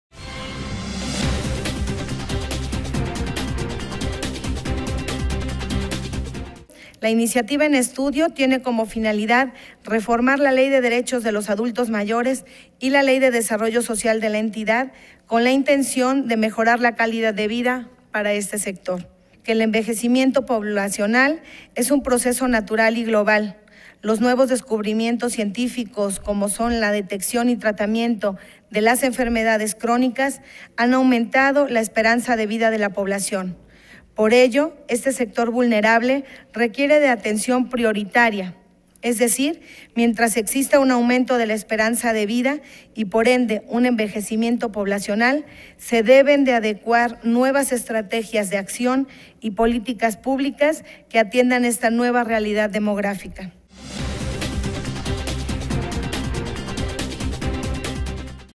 Erika Saab Reformas en favor de mejorar la calidad de vida de los adultos mayores 29 05 18
El envejecimiento poblacional es un proceso natural y global, los nuevos descubrimientos científicos como son la detección y tratamiento de las enfermedades crónicas han aumentado la esperanza de vida de la población, explicó la representante por el distrito de San Felipe Orizatlán, Erika Saab Lara.
Lo anterior, al dar lectura al dictamen aprobado que reforma la fracción V del artículo 4; artículo 5; fracciones IV, XI y XII del artículo 7; y la fracción VII del artículo 20 de la Ley de los Derechos de los Adultos Mayores del Estado de Hidalgo; así como a la fracción VIII del Artículo 9 de la Ley de Desarrollo Social para el Estado de Hidalgo.